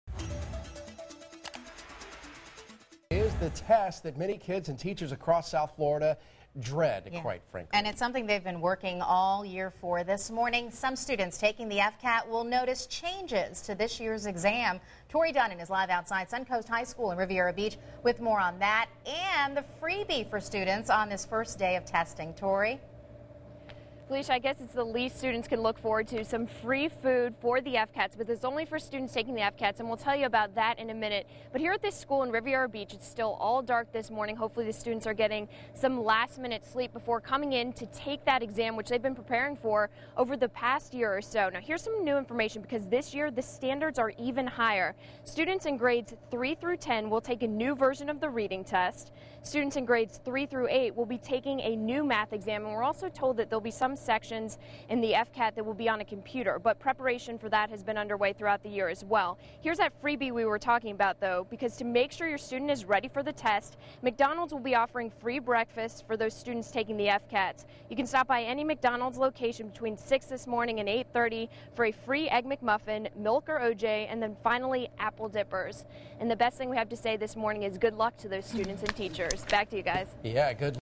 FCAT Begins Today
Some new changes are in place to this year's FCAT, which begins today.